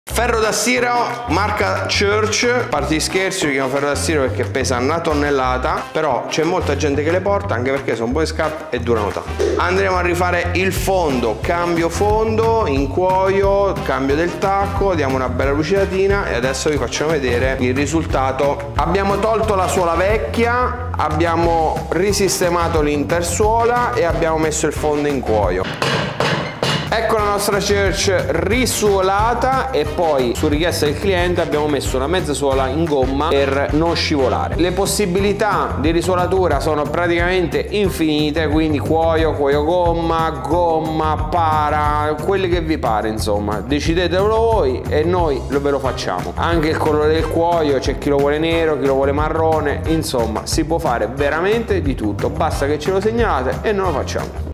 Un ferro da stiro della Church's
Oggi abbiamo un'altra pesantissima Church's con il fondo tutto usurato, però siccome la tomaia è di alta qualità, basta risuolarla e torna come nuova.

Vuoi avere un preventivo per una riparazione? Mandaci qualche foto su WhatsApp cliccando qui e ti diremo fattibilità, tempi e costi.

Dove siamo? A Milano in Corso di Porta Romana 40.